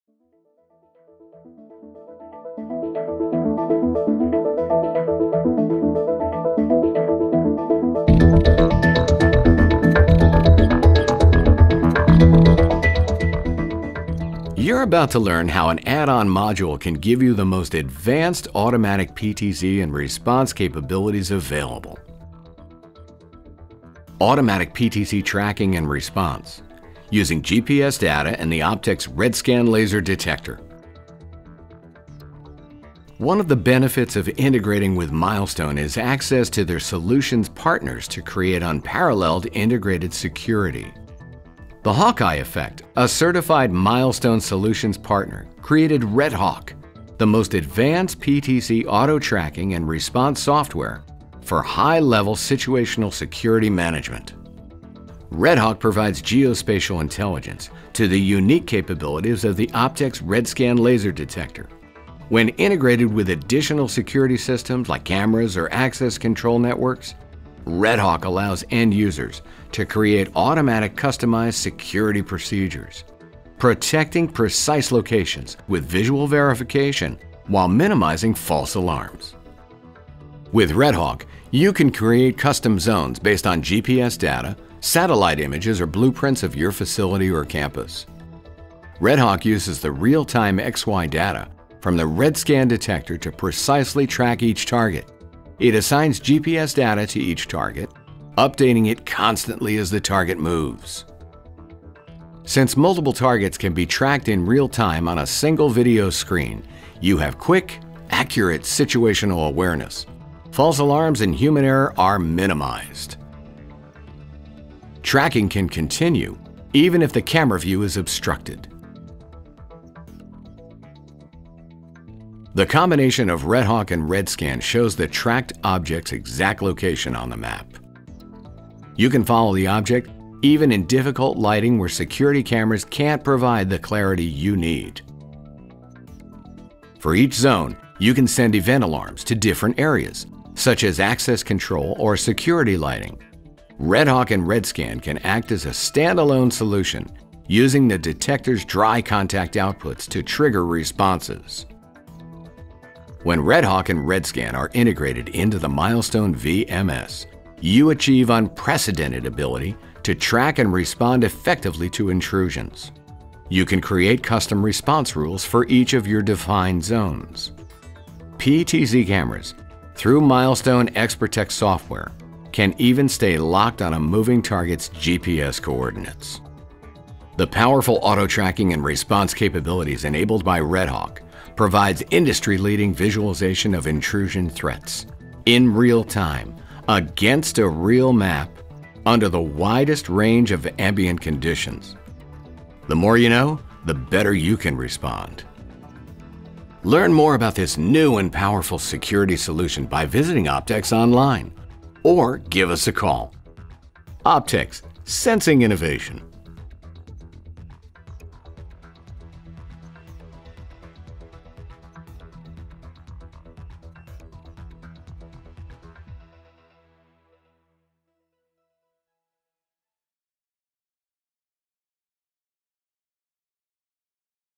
Optex Redscan PTZ Auto Tracking And Response using GPS
RedHawk software, created by Milestone Solutions Partner The HawkEye Effect, allows end users to protect precise locations by integrating user-determined security procedures with critical site information and exact target positions. RedHawk builds a custom GPS map providing users with heightened visual awareness of critical assets and key locations. RedHawk uses Target information from the OPTEX Redscan Laser Detector (RLS) to gather accurate information about Target position in relation to these key locations. When a Target enters a protected area, RedHawk automatically initiates the custom security response.  In conjunction with Milestone XProtect, PTZ camera networks, and other critical response systems like Access Control or IP relays, RedHawk provides a variety of layered security options automatically, without the need of human involvement.